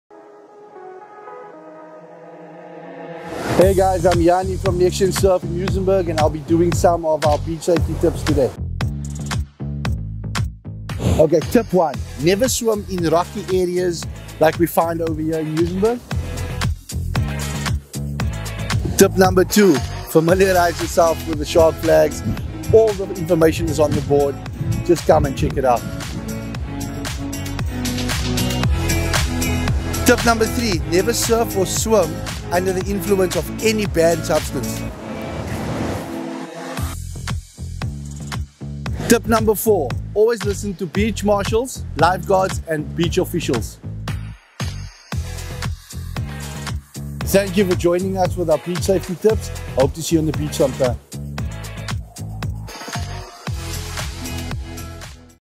TravelWise - Beach safety
February is one of Cape Town's best beach going months. Here are some helpful beach safety tips from @nexgensurf - Thank you @yanitrout for sharing.